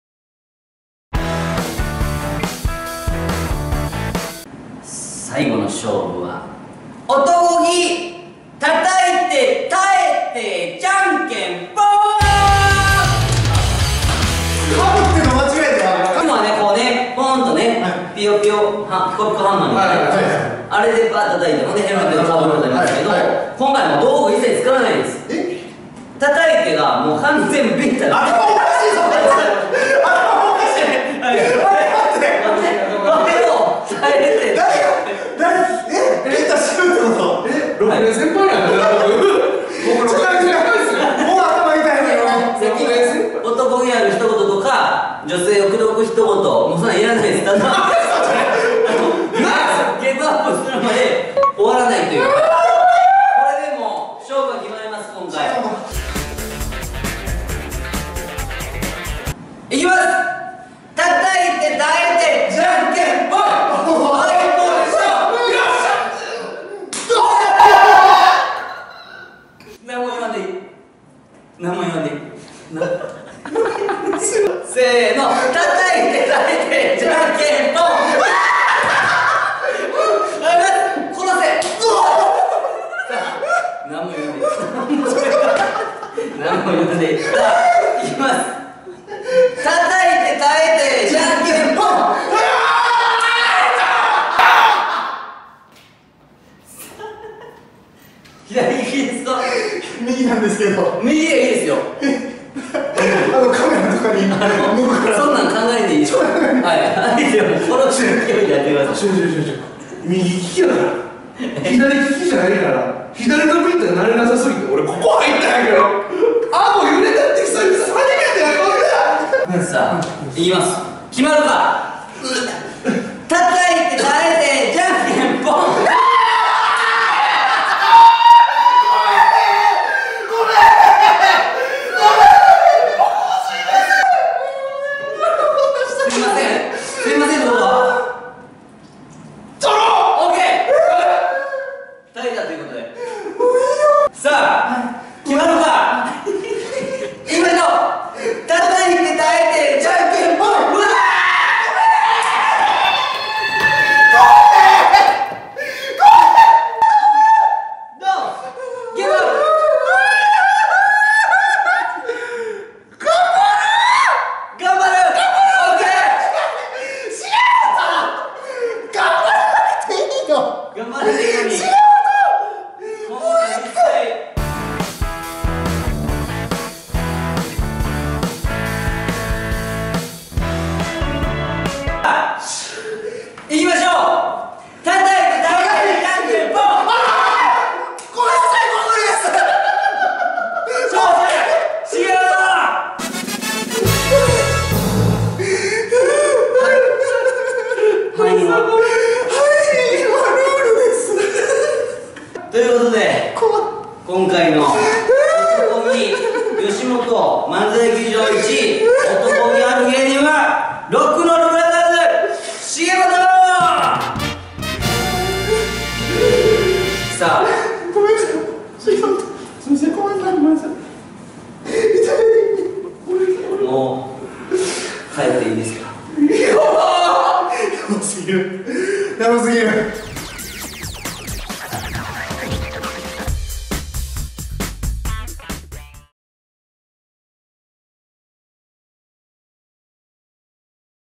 衝撃！「男気対決！男らしヘンダー！」＃３（完）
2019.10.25ヘンダーソン単独ライブ「HENDER×HENDERvol13」
ブリッジVTR①

ヘンダーソン子安が“真の男”の称号をかけてゲストと男気対決！